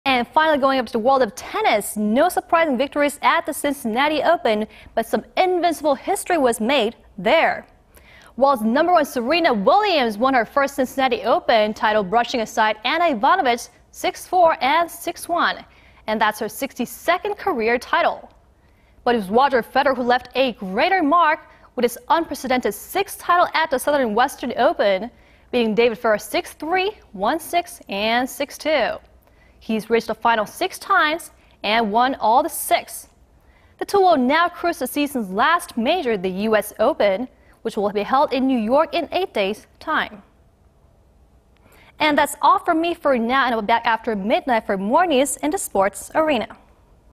Tennis: Federer, S.Williams win Cincinnati Open
Now going over to world of tennis... no surprising victories at the Cincinnati Open but some invincible history was made there.
World's number one Serena Williams won her first Cincinnati Open title brushing aside Ana Ivanovic 6-4, 6-1. That's her 62nd career title.
But it was Roger Federer who left a greater mark... with his unprecedented sixth title at the Southern and Western Open... beating David Ferrer 6-3, 1-6, 6-2.
He's reached the finals six times and won all six.
The two will now cruise to the season's last major,... the U.S. Open,... which will be held in New York in eight days time.
That's all from me for now and I'll be back after midnight, for more news in the sports arena.